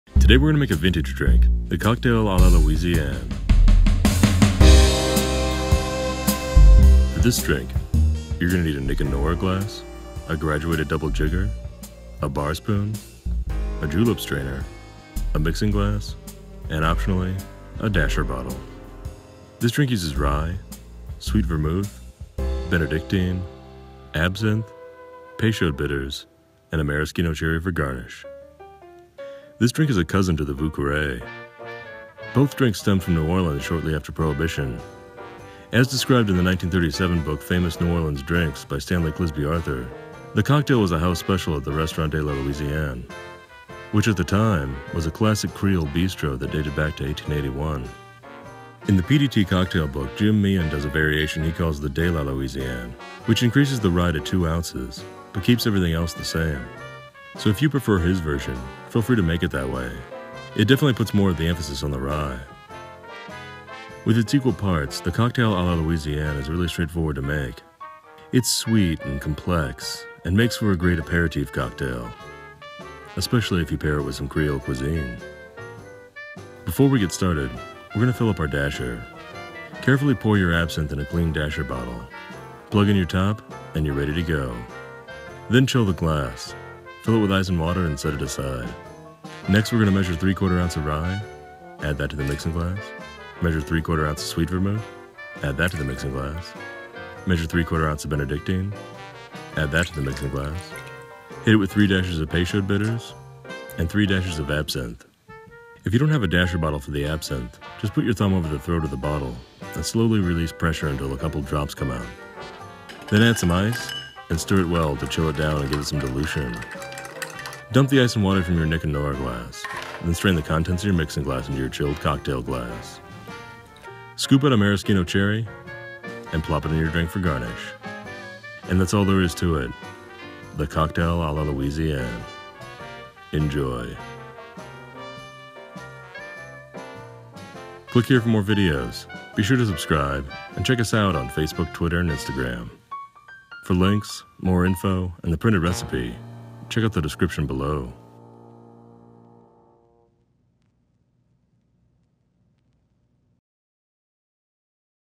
Cocktail à la Louisiane - How to Make the Classic New Orleans Cocktail
This Creole New Orleans classic drink is made with Rye Whiskey, Sweet Vermouth, Benedictine, Absinthe, Peychaud's Bitters and a Maraschino Cherry for garnish. The recipe was first published in Famous New Orleans Drinks and How to Mix Them by Stanley Clisby Arthur in 1937, along with another post-prohibition New Orleans classic, the Vieux Carré

According to Famous New Orleans Drinks, the cocktail was the house special at Restaurant de la Louisiane, which at the time was known for it's Creole cuisine. The Restaurant de la Louisiane had been around since 1881. Some time shortly after Famous New Orleans Drinks was published, the restaurant started a series of changing hands that steered the food away from its original Creole roots.

I departed slightly from my usual spirit suspects. I used a bottle of Sazerac Rye, which seemed fitting for a New Orleans drink, and a bottle of Copper & Kings Absinthe Blanche. You can certainly use your Absinthe of choice, my go-to is St. George Absinthe Verte, but the Copper & Kings is particularly mellow, which is good in a drink like this. It's just enough to give it a touch of anise and funkiness, but not enough to muscle out the other ingredients.

This is the original recipe that features equal parts, Rye, Sweet Vermouth and Benedictine. Jim Meehan, however, in his book, The PDT Cocktail Book, created a variation on this drink he referred to as "De la Louisiane". The only difference is that Meehan's version uses 2 ounces of Rye, everything else stayed the same. Meehan's version is stronger, boozier and certainly puts the emphasis on the Rye.

I certainly like Meehan's, so feel free to make it that way if you're in the mood, but I tend to like the original especially if it's the first cocktail of the night. Either way, you'll have a spirit-forward, sweet-leaning, aperitif cocktail that works particularly well before a meal of Creole cookin'. Who dat!

Recipe:
0.75 oz Rye Whiskey
0.75 oz Sweet Vermouth
0.75 oz Benedictine
3 dashes Peychaud's bitters
3 dashes Absinthe
garnish Maraschino Cherry

Stir with ice in a mixing glass. Strain into a chilled cocktail glass. Garnish with a cherry.

De la Louisiane (Jim Meehan's) Recipe:
2 oz Rye Whiskey
0.75 oz Sweet Vermouth
0.75 oz Benedictine
3 dashes Peychaud's bitters
3 dashes Absinthe
garnish Maraschino Cherry

Peychauds Bitters

Luxardo Maraschino Cherries

Bar Tools:

Nick & Nora Glass

OXO SteeL Double Jigger

Julep Strainer

Mixing Glass

Bitters Dasher